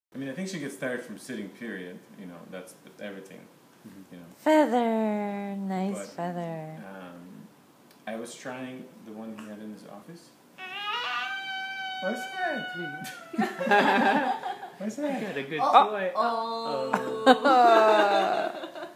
Two handed self directed play with Wrex
Aviv enjoys playing with a feather.